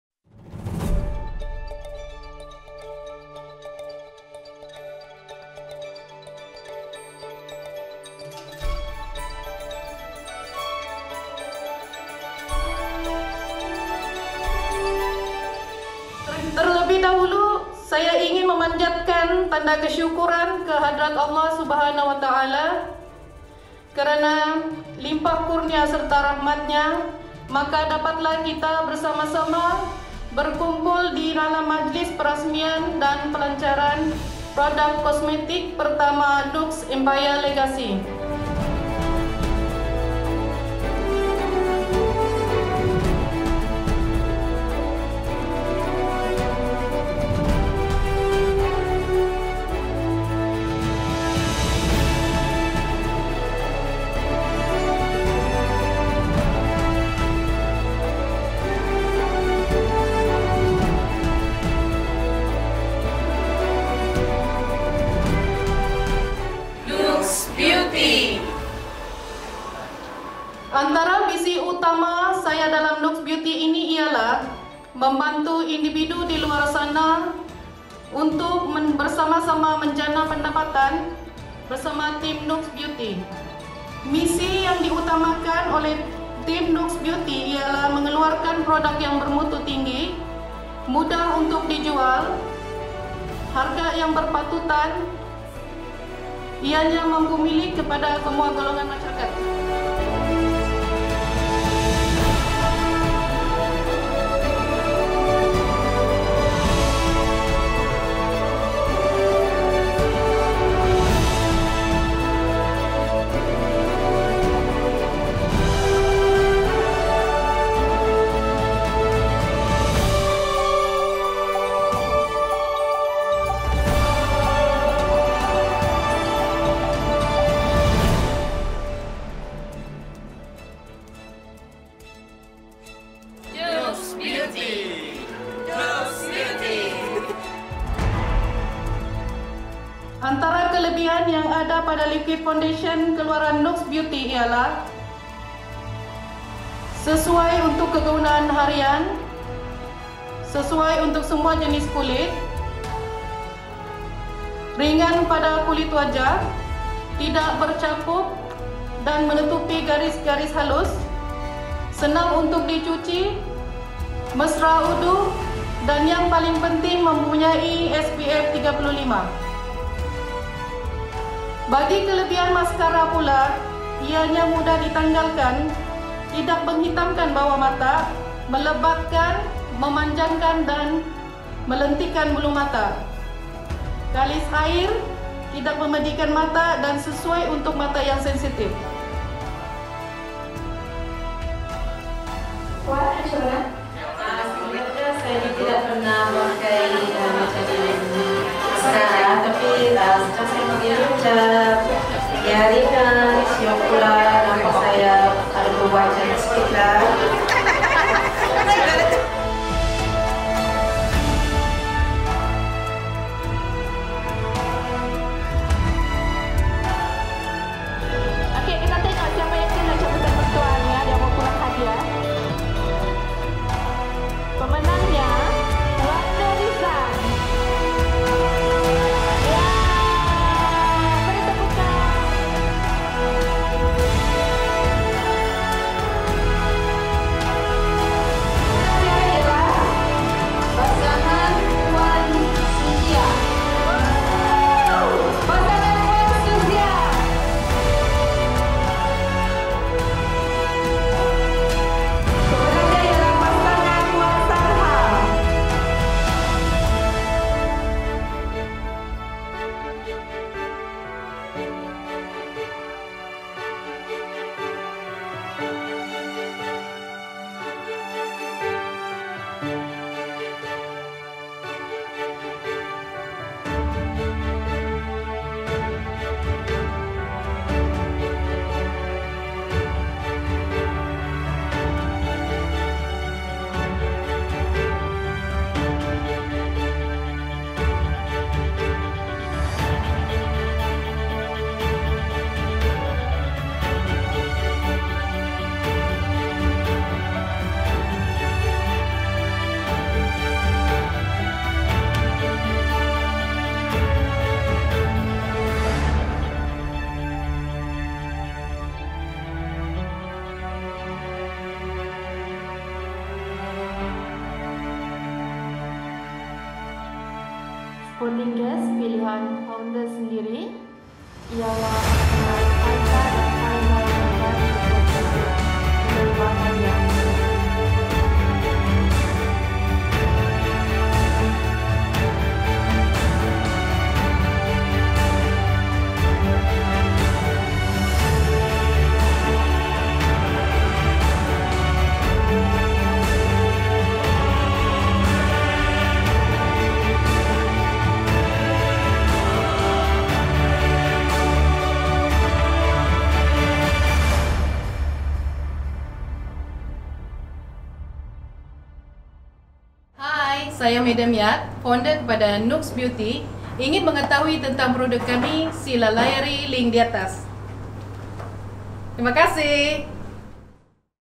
NUKS Beauty Grand Launching
New product launching from NUKS Beauty at Lintas Platinum Hotel.